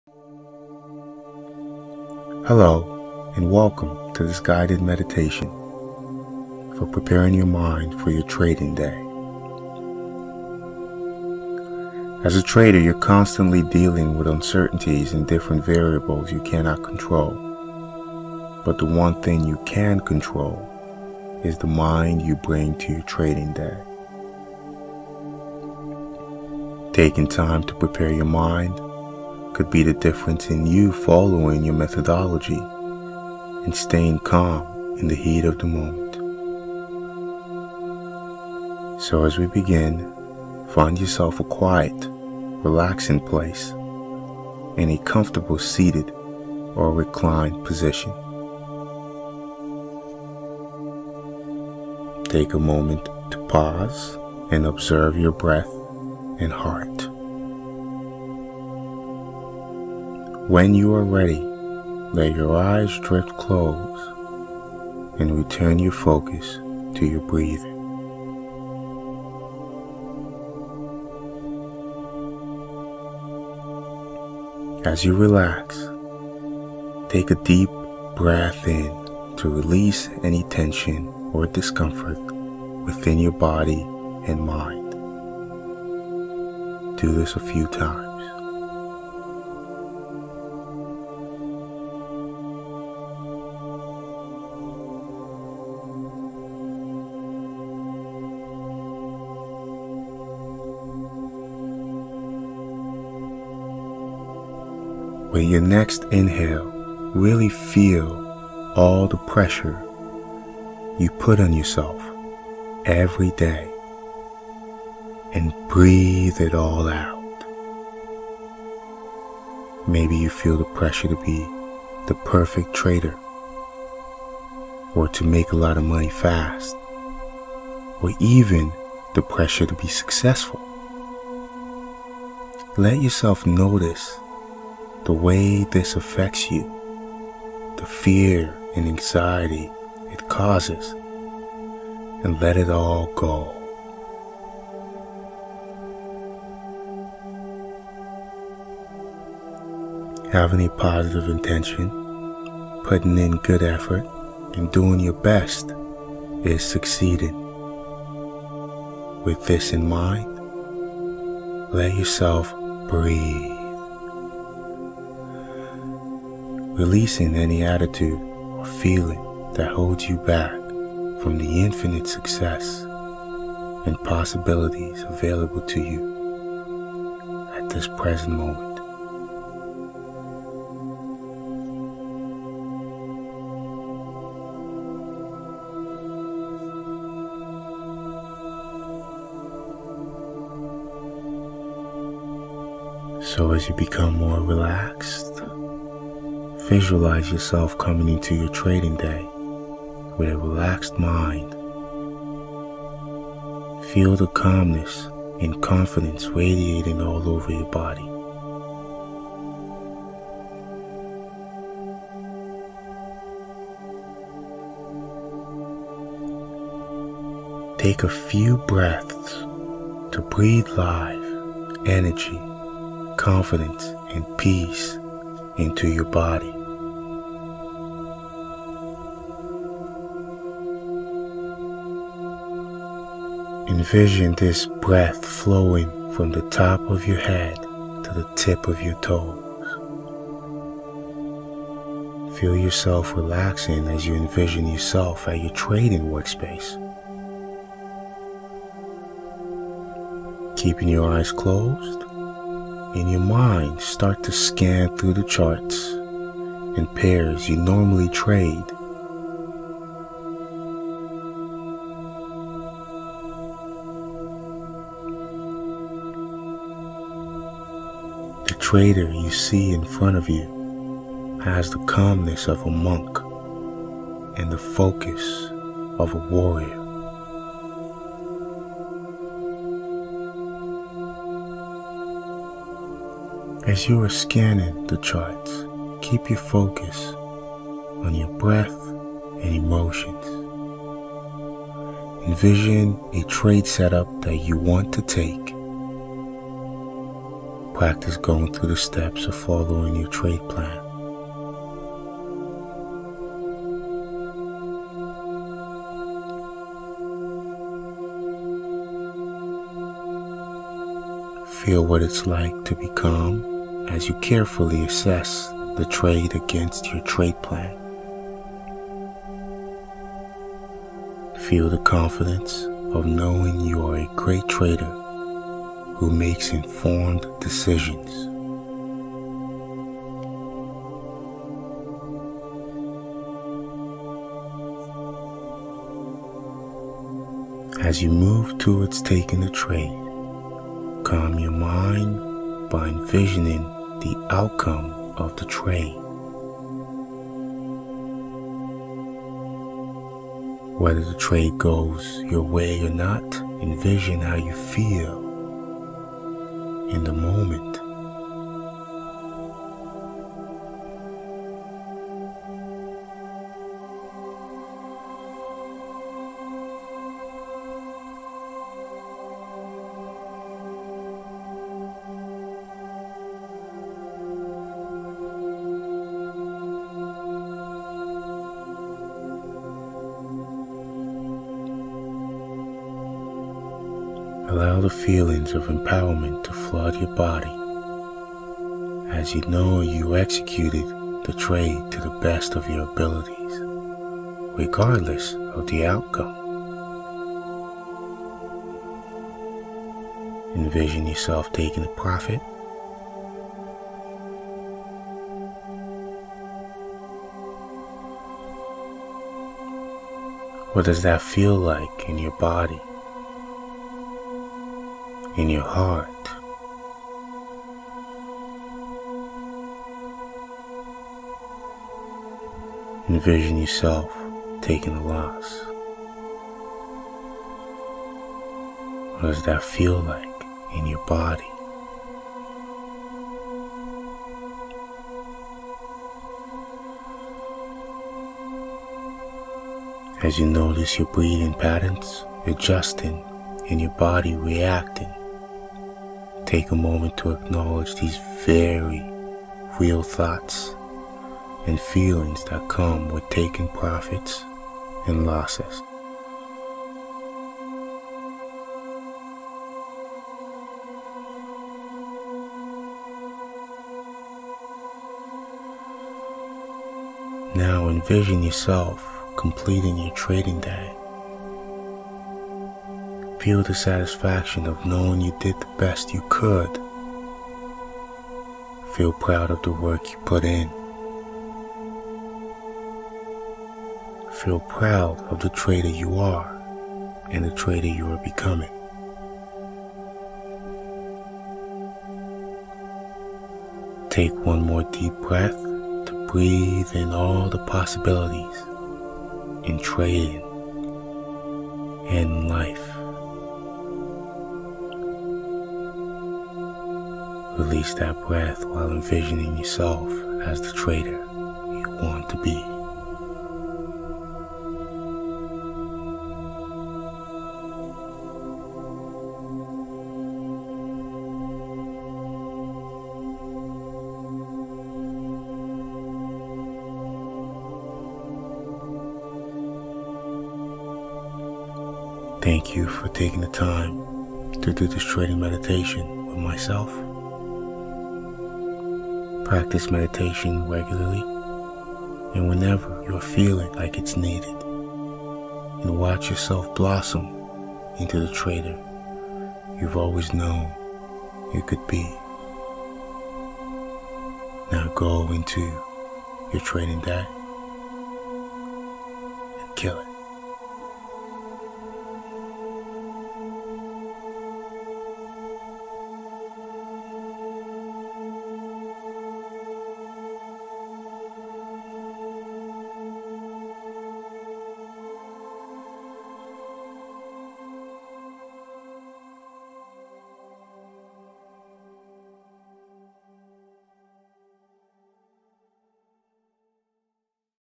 Morning Meditation For Traders 10 Minutes - Optimize Your Performance
Morning meditation for traders to help optimize your performance in trading. It'll also help you cultivate a more relaxed and focused mind, improving your decision-making process and allowing you to remain calm in the heat of the moment.

Learn how to become a successful day trader today!